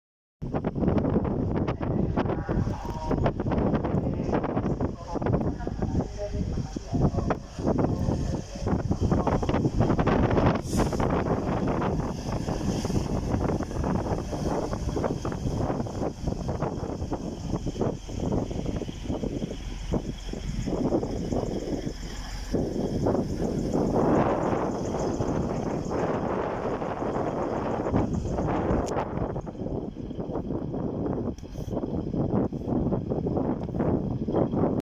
185103 arriving into Doncaster on 1B79
185103

185103 Coaches:51103, 53103, 54103

1B79 12:26 Cleethorpes - Manchester Airport